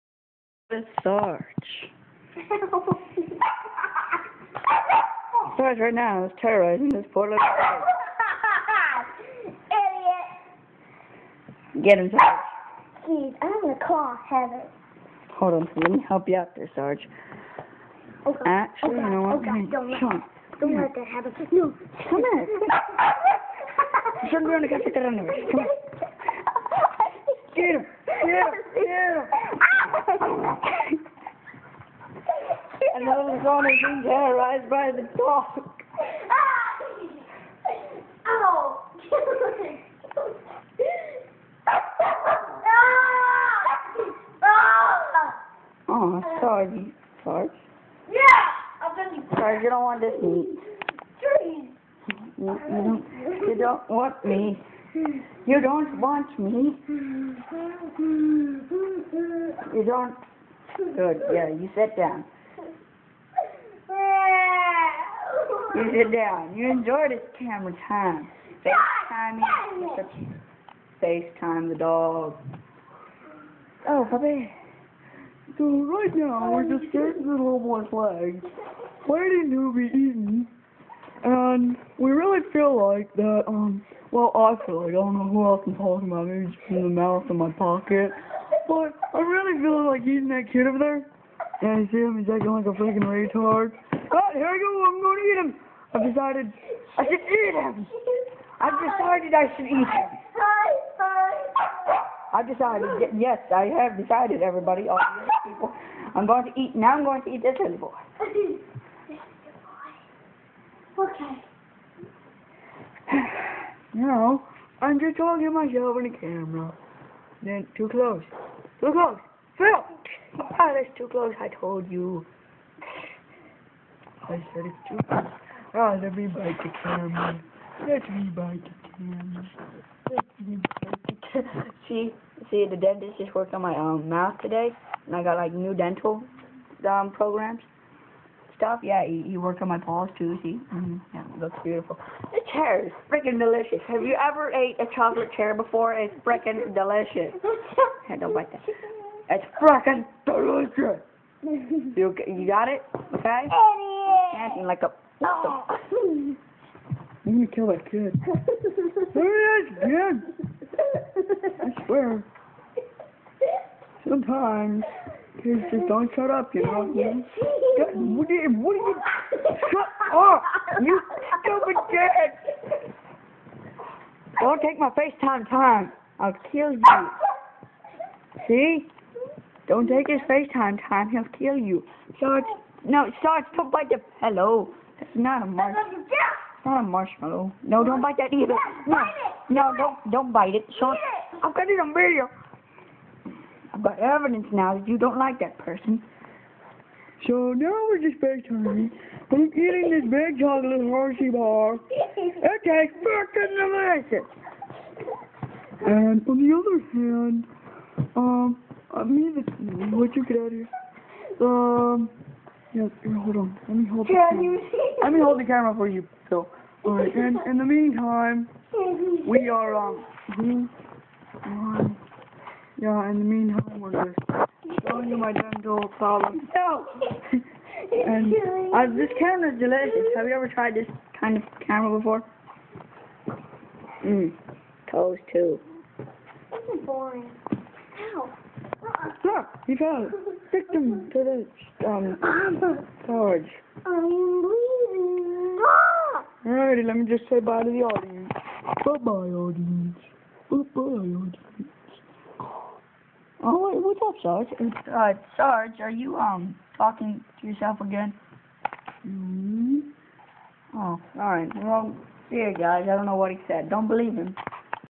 Kid gets owned by dog
My little brother get owned by sarge my german sheperd puppy